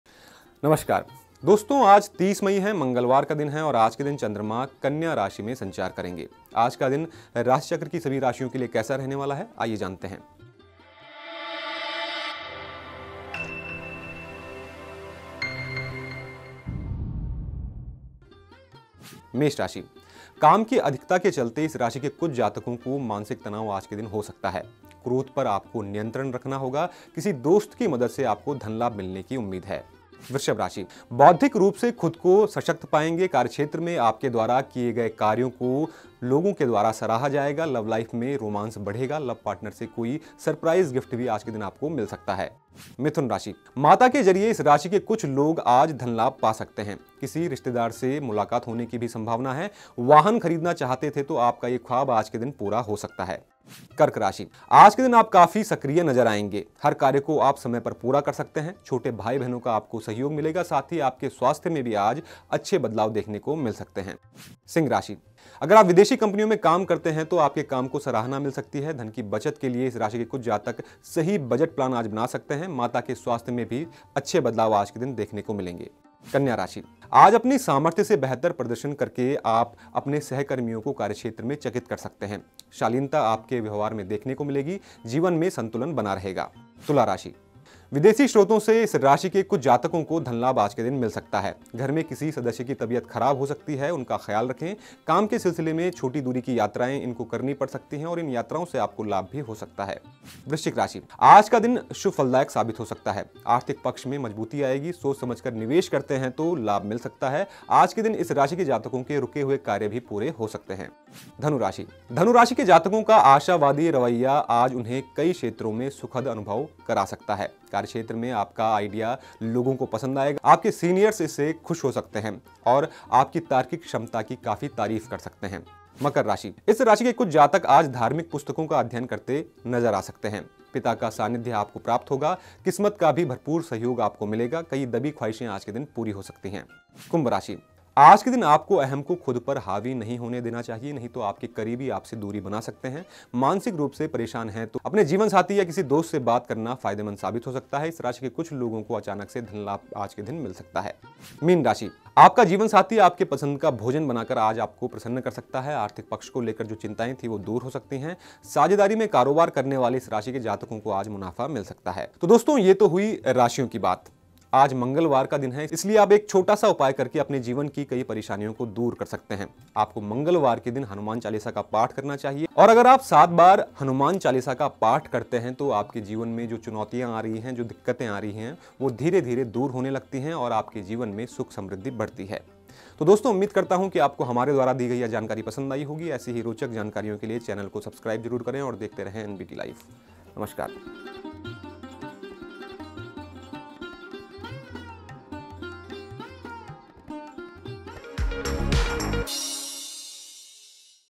Aaj Ka Rashifal 30 May मंगलवार का दिन इन 3 राशियों के लिए रहेगा शुभ| Today Horoscope | NBT Life
आज 30 मई है शनिवार का दिन है और आज के दिन चंद्रमा सिंह राशि में संचार करेंगे। आज का दिन राशिचक्र की सभी राशियों के लिए कैसा रहने वाला है आइए जानते हैं।